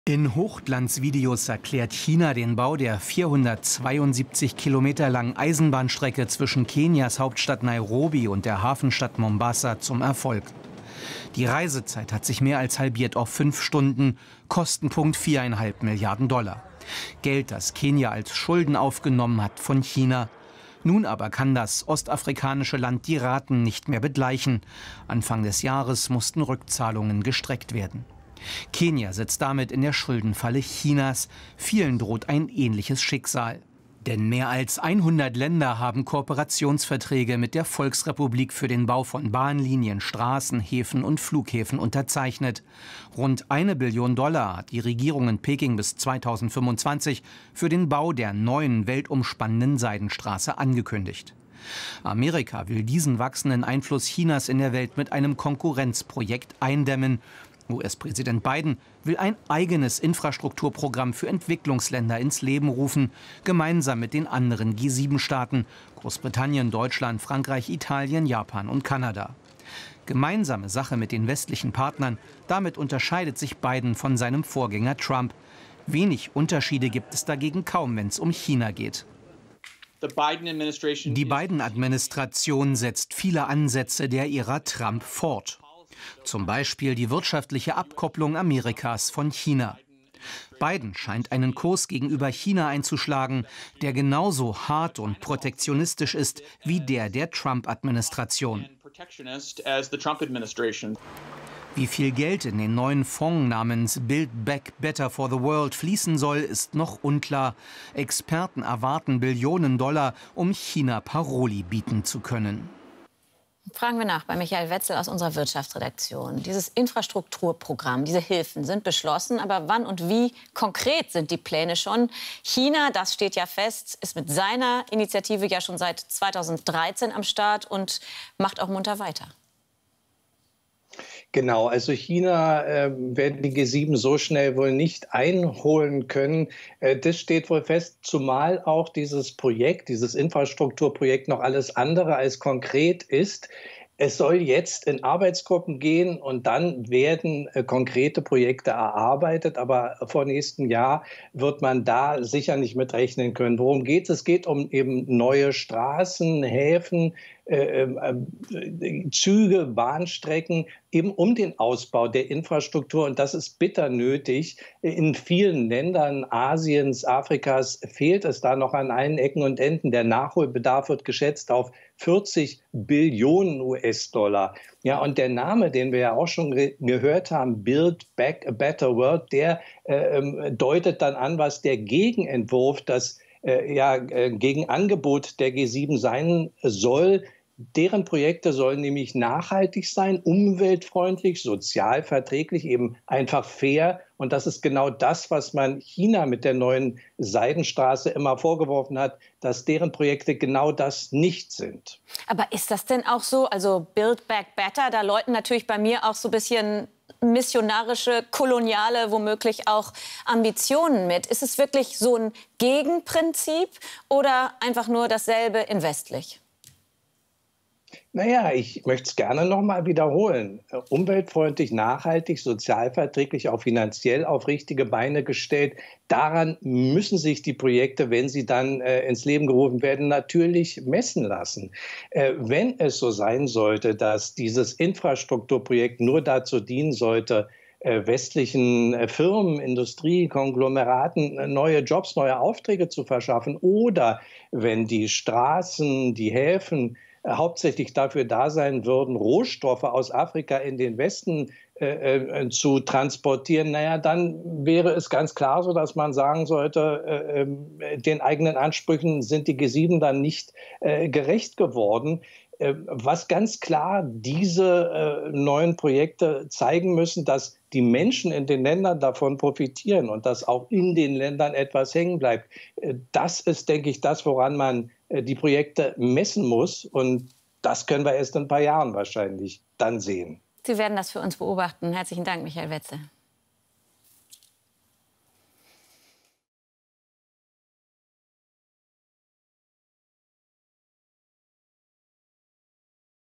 Kann der G7-Infrastrukturplan Chinas Seidenstraße kontern? | DW Nachrichten
Militärisch wie wirtschaftlich bastelt der US-Präsident an Wegen und Allianzen, um China die Stirn zu bieten. Dabei hat Joe Biden mit den sechs anderen führenden Industriestaaten und der EU am Wochenende beschlossen, Milliarden zusätzlicher Infrastrukturhilfen für Entwicklungsländer bereit zu stellen. Das soll eine Antwort auf Chinas Langzeit-Projekt der Neuen Seidenstraße sein.